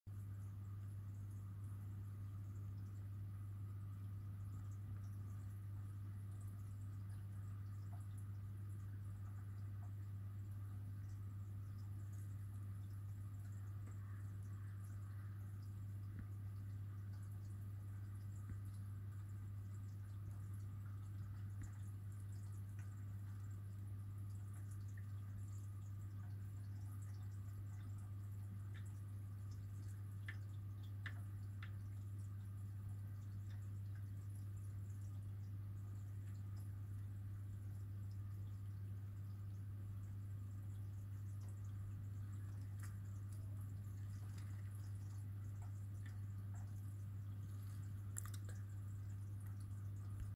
50 second time-out for your day 🐡🐟🐠
Take a 50 second time-out (if you want!) to watch a convict tang and coral beauty pecking at some nori - it can be relaxing! :D

This is after the initial period the food has been offered because the tang goes into a frenzy as soon as the nori hits the water - some might think he's gone weeks instead of a few hours without food! Very greedy fish is your average convict tang! ;-)

As always see you out there and on YouTube - Snowymatrix